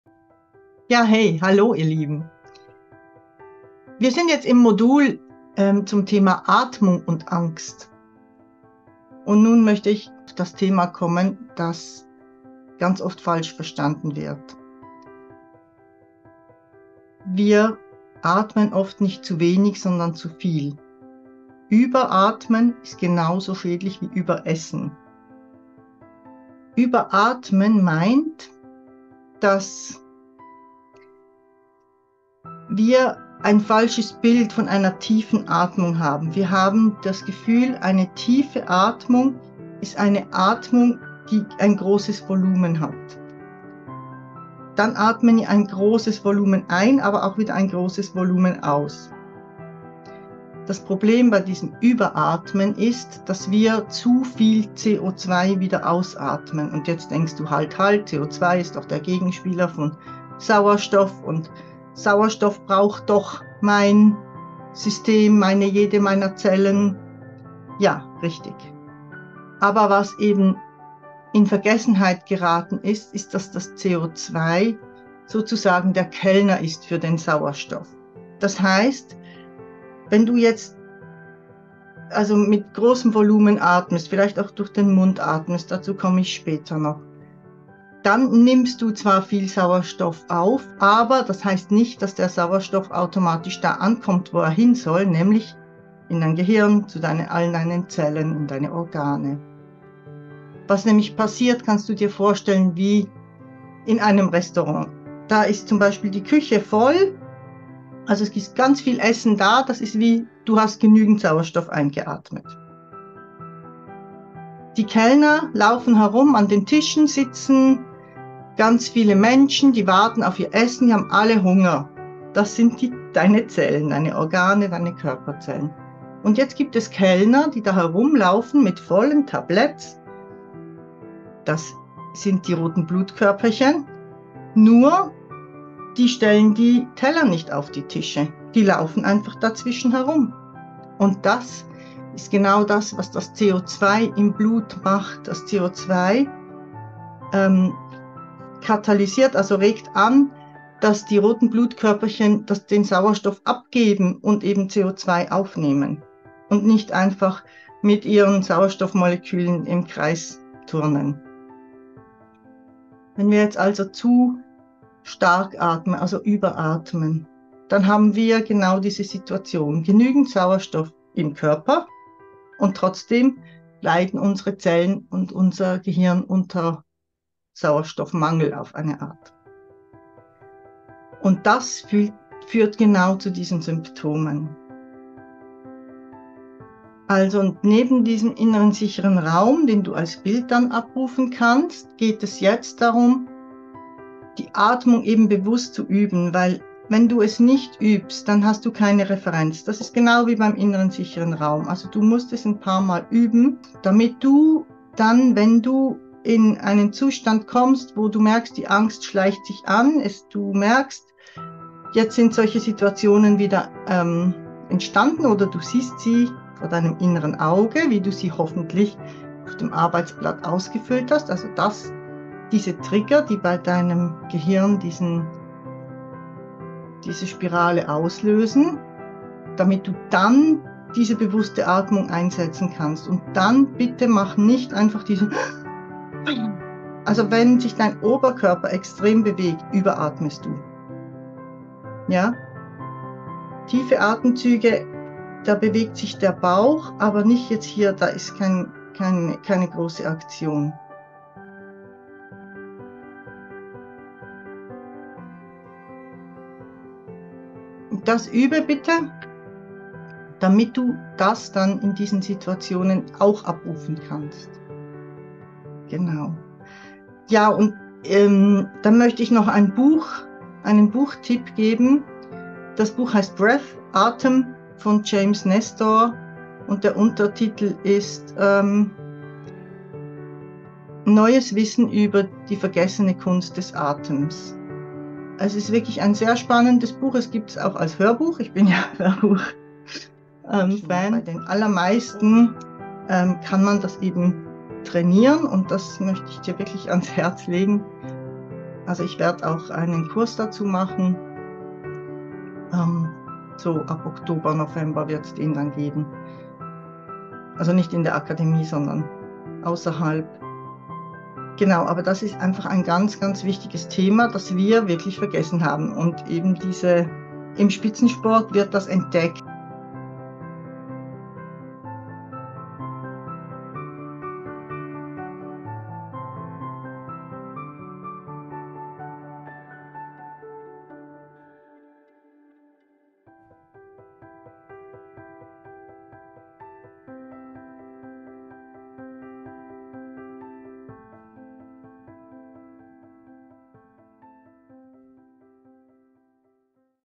Angst und Atmung - unser häufigster Fehler
Hier findest Du einen kleinen Ausschnitt aus dem Modul zur Atmung und zur Angst aus unserem umfassenden Kurs, der Dir hilft, Deine Angst beim Pferd und/oder beim Reiten zu meistern.

Darin findest Du Grundlagen zum Verständnis der Angst, Skills um sie anzugehen eine konkrete Anleitung zum Aufbauen der Sicherheit vom Boden aus und beim Reiten. Ohne in die Dominanz zu gehen.